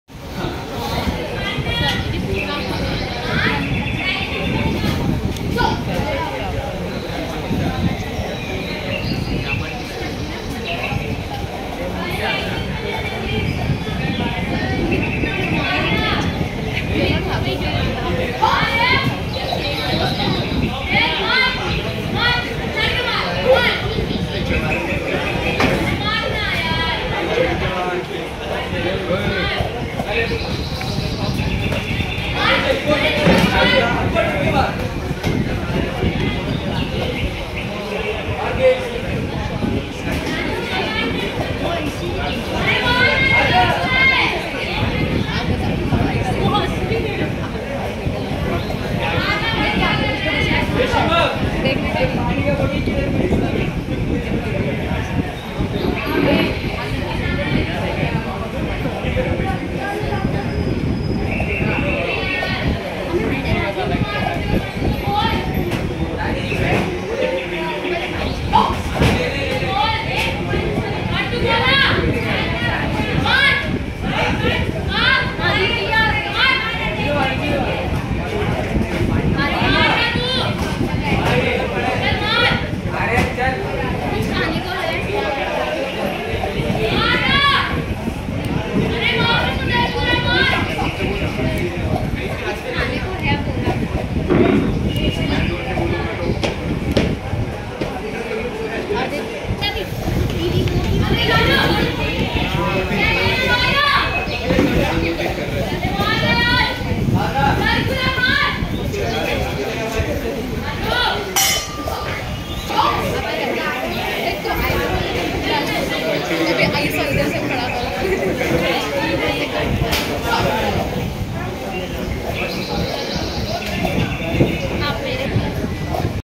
Boxing :- 34th Junior Inter College Boxing Championship 2022-2023, 18th April 2023, Prakash College
Boxing :- 34th Junior Inter College Boxing Championship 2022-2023
Tournament Date :
Tuesday, 18th April 2023
Venue : Prakash College, Kandivali, (West) Mumbai

The main aim of this video is to promote Sports in all over Mumbai, Maharashtra and India in Cities and Villages.